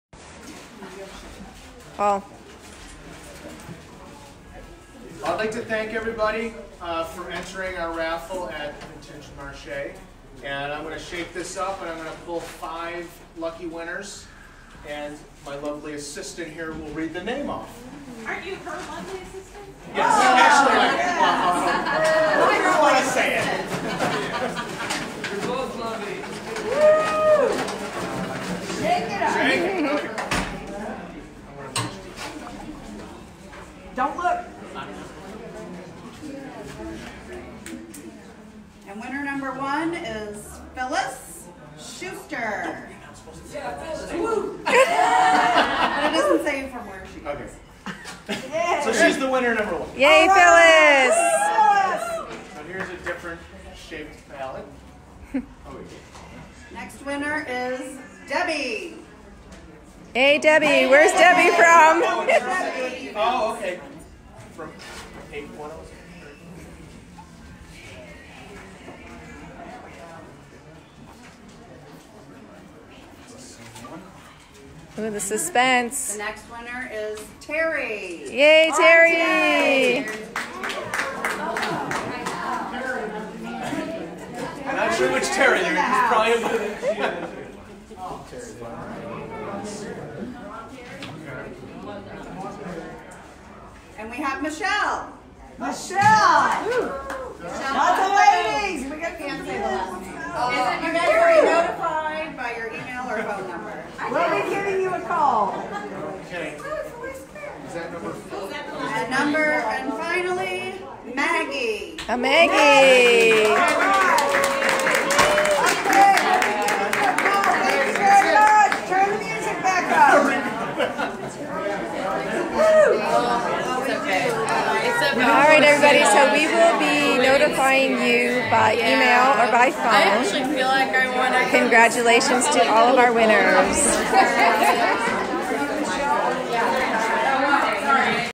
January Market Gift Card Winners
We were so excited at Vintage Marche for our first market in January of 2020.  We had a drawing for five $100 Gift Cards to celebrate.  We have so much fun at Vintage Marche!